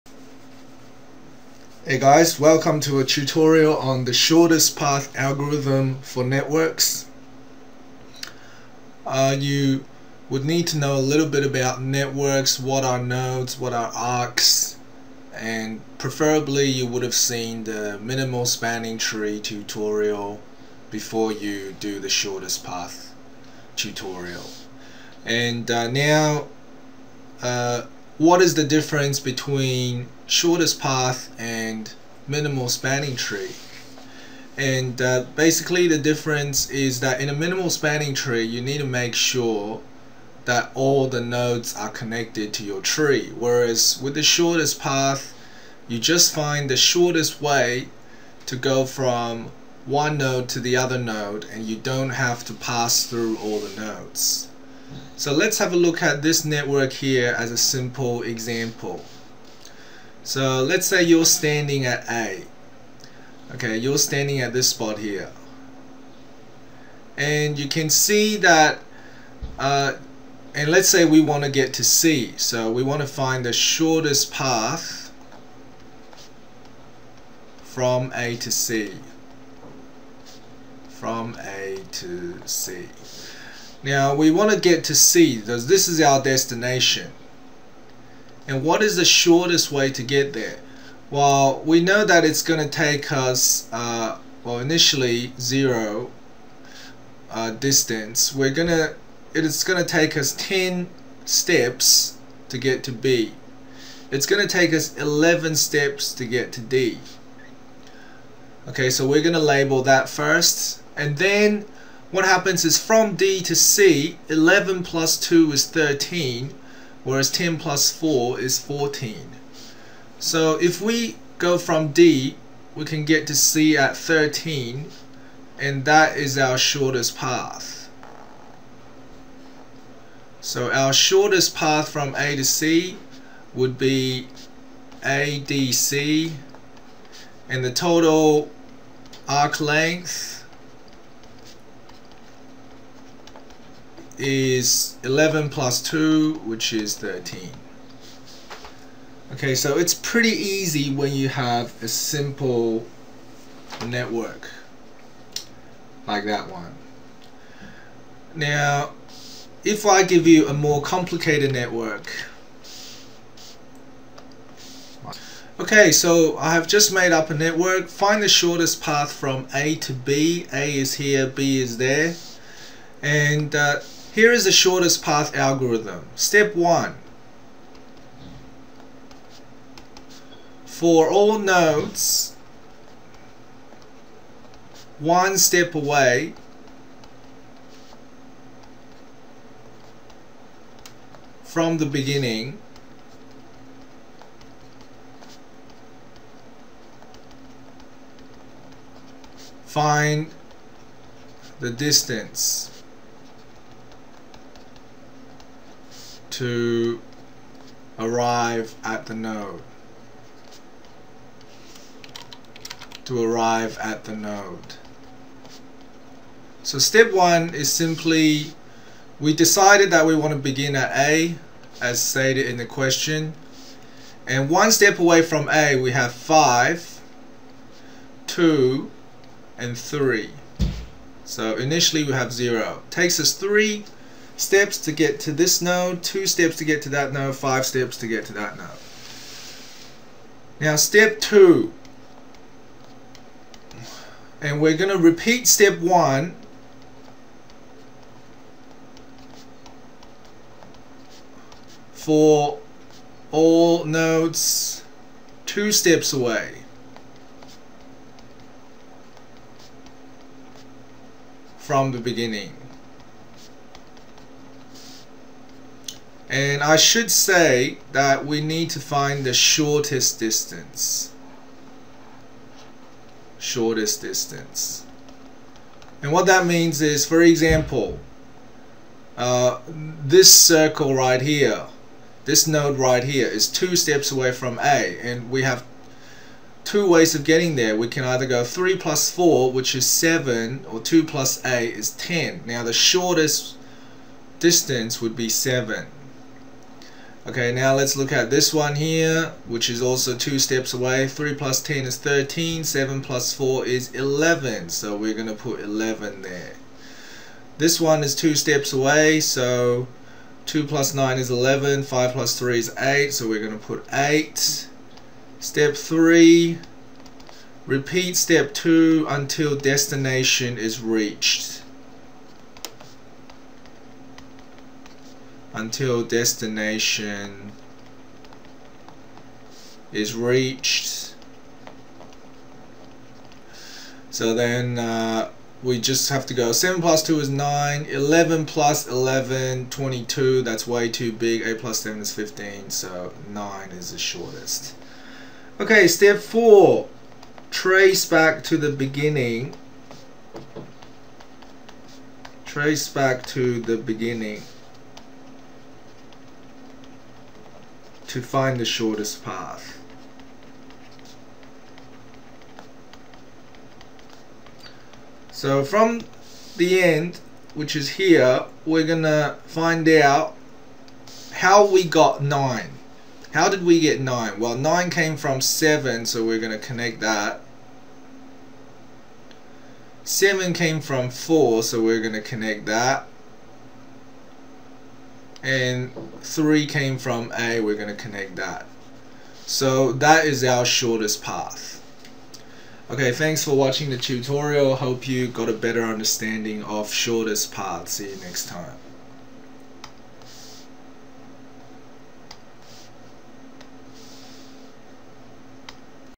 Networks - Shortest path algorithm
In this tutorial you learn about how to draw the shortest path between two nodes using the shortest path algorithm.

This topic is taught in Queensland Maths A, Year 11 or Year 12.